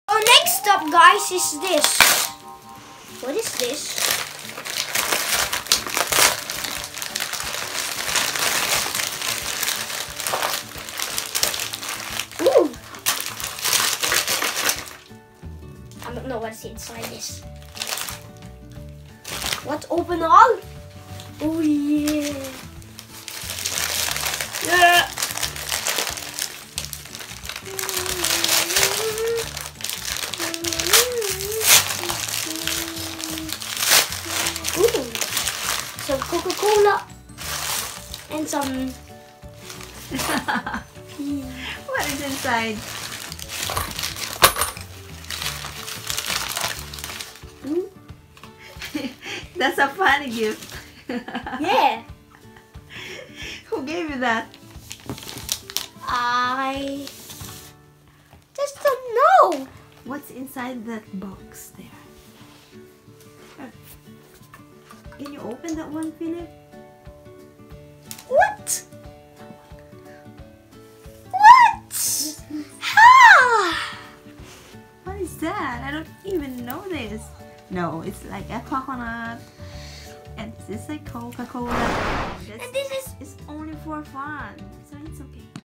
POLITE REACTION TO A PRANK CHRISTMAS Gift
trying to be polite even he wanted to complain😆😂